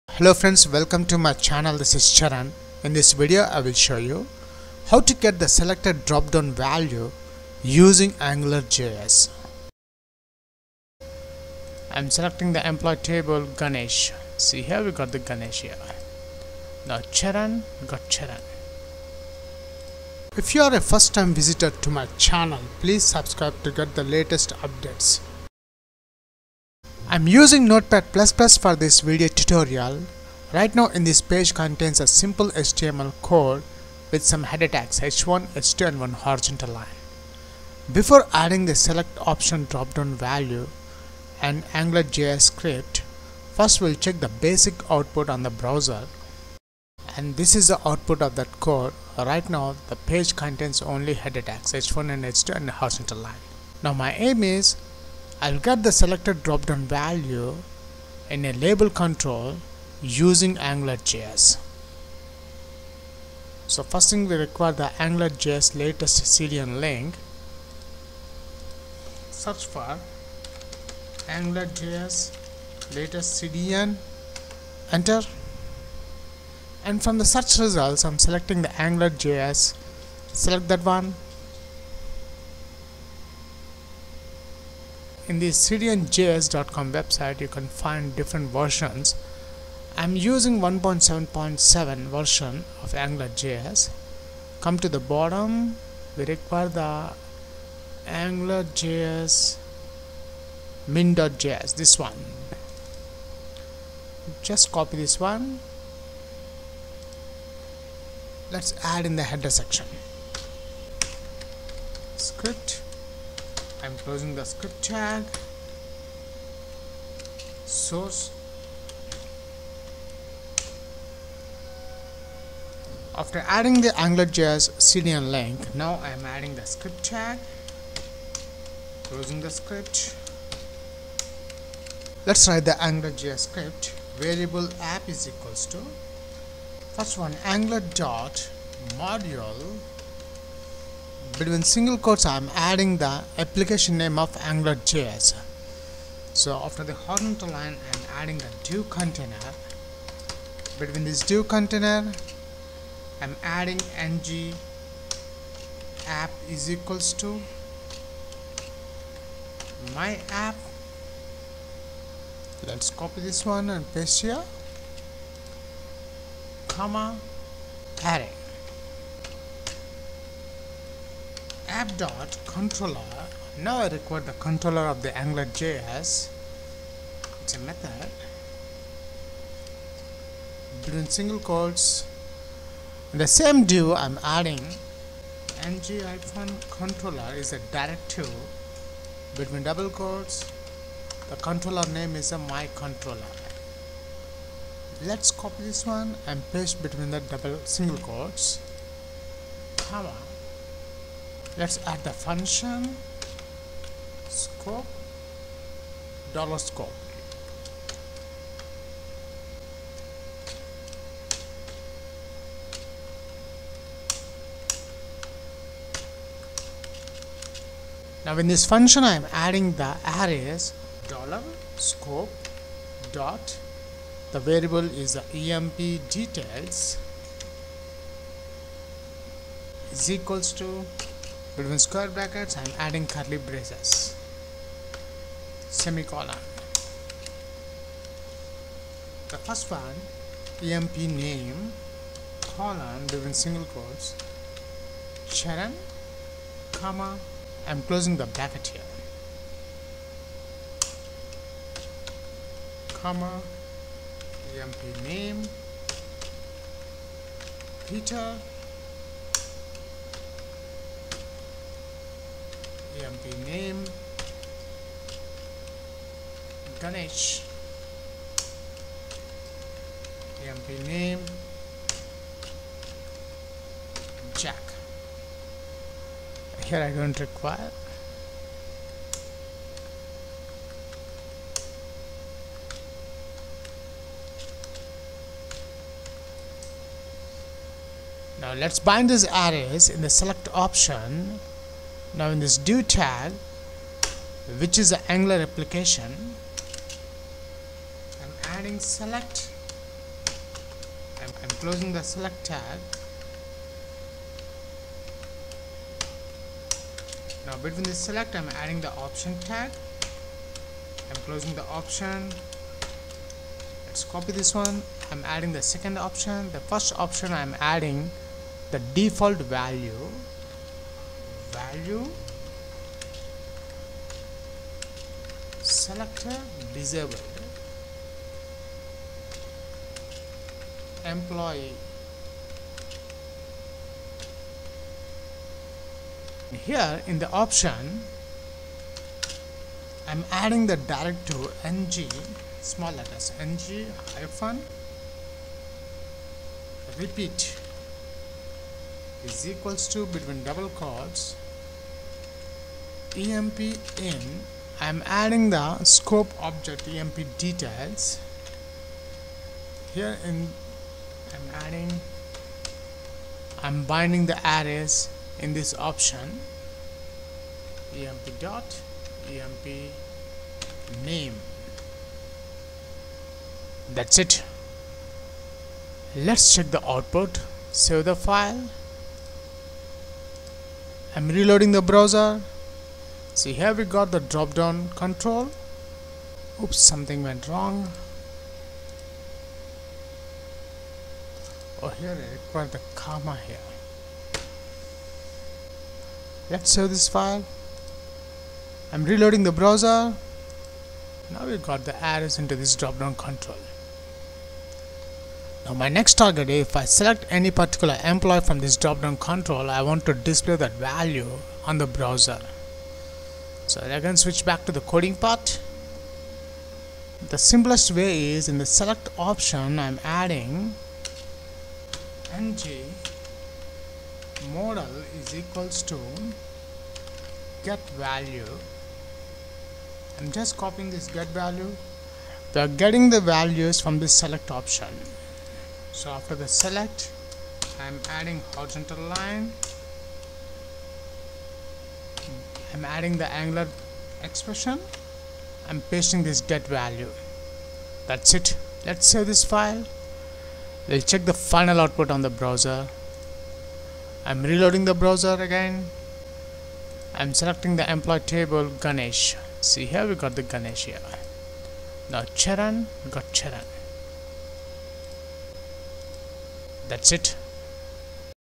get selected dropdown (select option) value in angularjs 1.7.7
angular js tutorial get and display the selected dropdown value select option html element